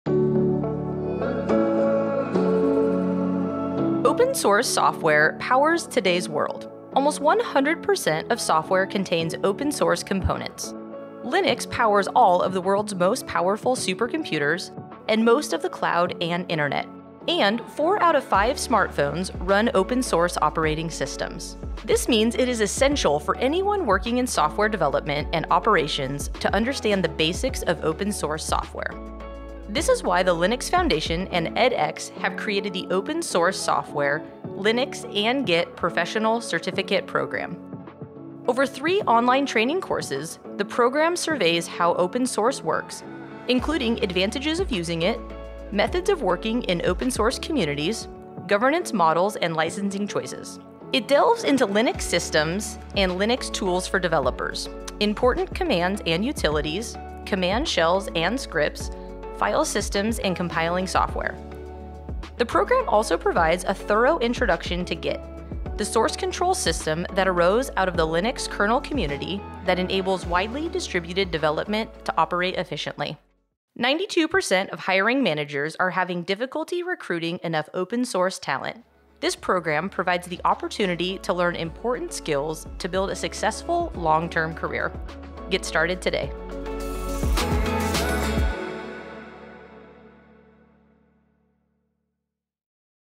Professional Certificate in Open Source Software Development, Linux and Git
The “Open Source Software Development, Linux and Git” Professional Certificate Program is designed for developers with experience working on any operating system who want to understand the basics of open source software development. The program surveys how open source software works, including advantages of using it, methods of working in OSS communities, governance models and licensing choices. It delves into Linux systems and Linux tools for developers, including installation, desktop environments, text editors, important commands and utilities, command shells and scripts, filesystems and compiling software. It also provides a thorough introduction to Git, the source control system that arose out of the Linux kernel community, that enables widely distributed development to operate efficiently.